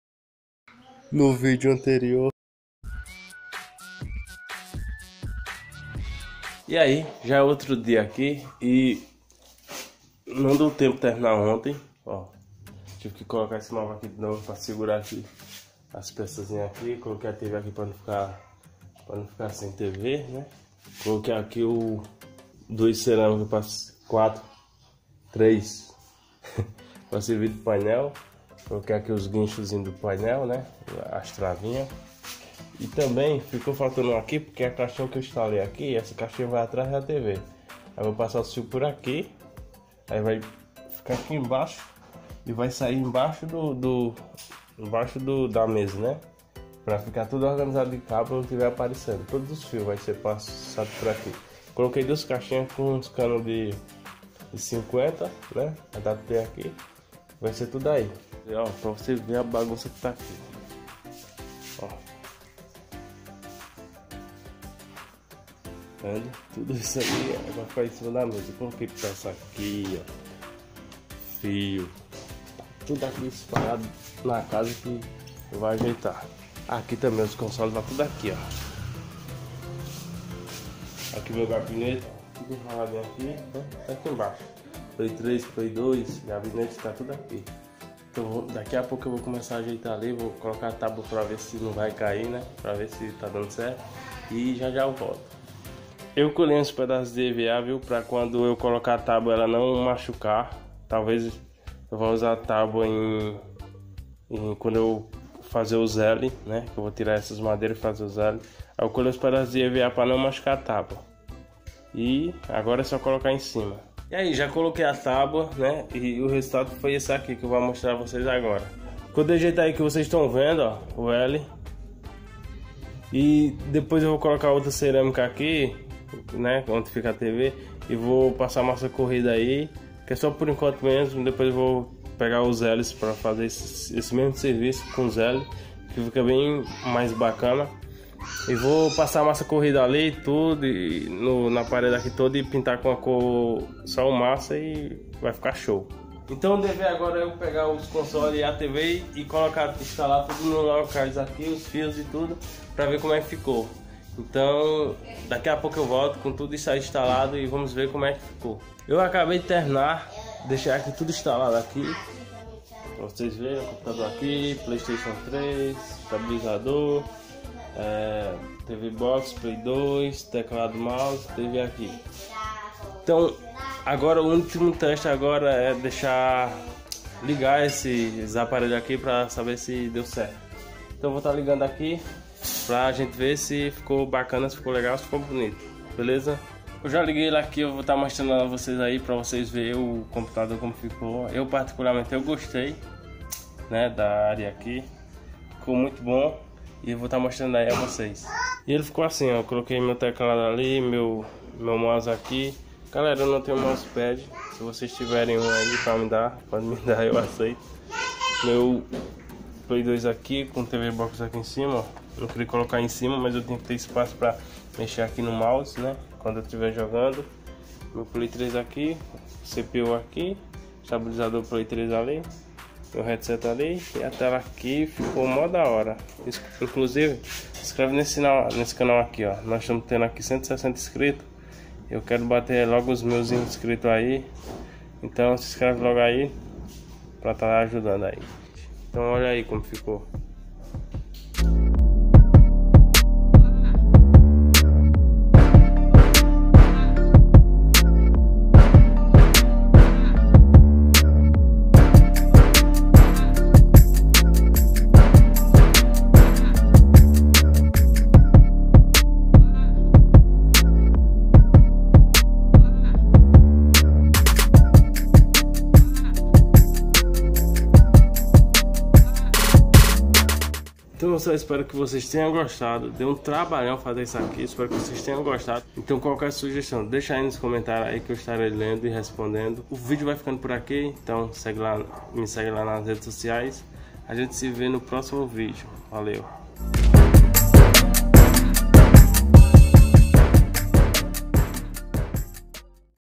PROJETANDO MESA PARA PC PARTE #2
Nesse vídeo que é a parte 2 estou projetando uma mesa para computador.
Não é nada profissional 😜, é só uma maneira criativa para vc fazer uma caso não tenha ...